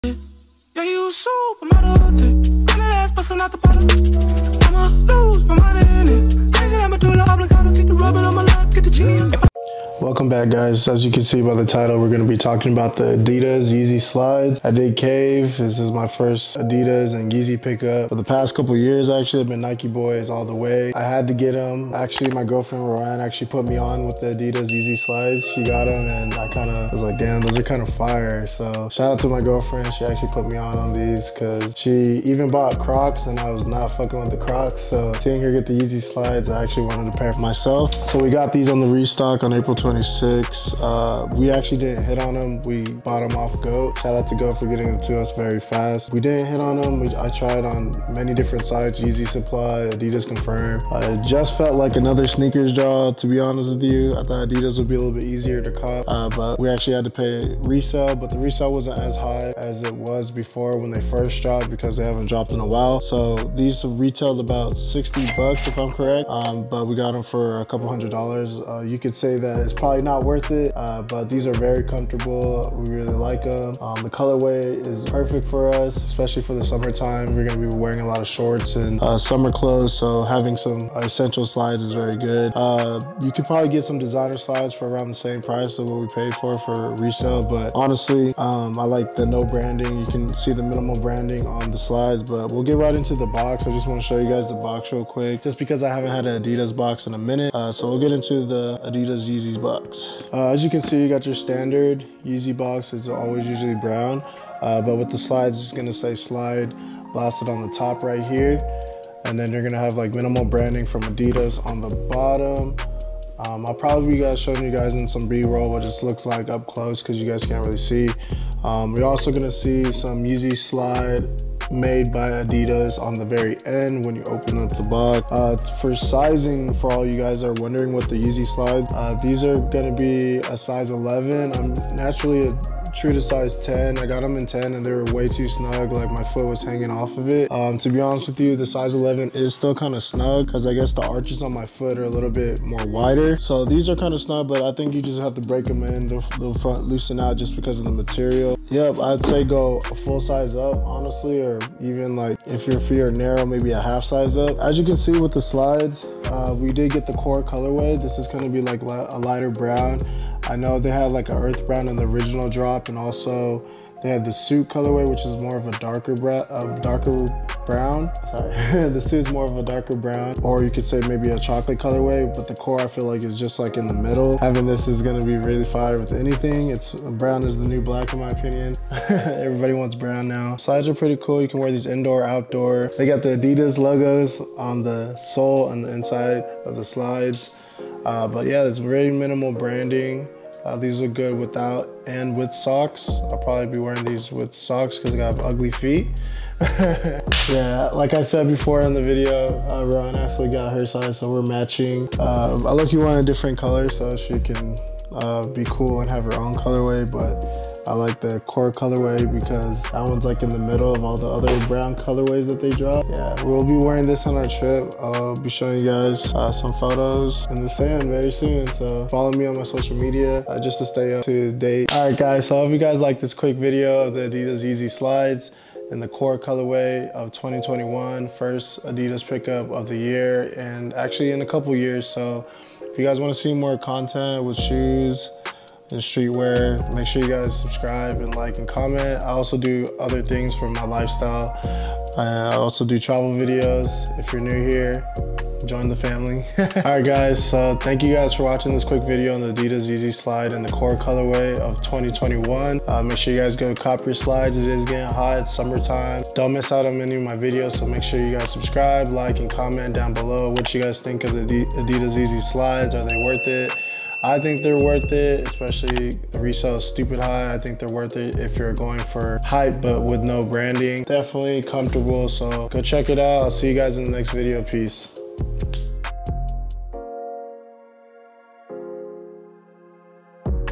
Adidas Yeezy Slide "Core" 2021 Review (On Feet, + Sizing)
Adidas Yeezy Slide "Core" 2021 Review with on feet and sizing.
I'll be showing you what they look like in hand and letting you get a close up look at the slides. Pricing and honest opinion are also included in this video.

Official Name: Adidas Yeezy Slides "Core" 2021
Product Description via Goat App: The adidas Yeezy Slide ‘Core’ showcases a monochromatic light brown hue throughout the exterior of the futuristic sandal, featuring open-toe construction on a cushy EVA foam build. Branding is limited to adidas’ three-stripe logo stamped on the footbed and a Yeezy wordmark on the lining of the upper. Underfoot, the outsole is treated to a series of deep horizontal grooves for soft cushioning and enhanced traction.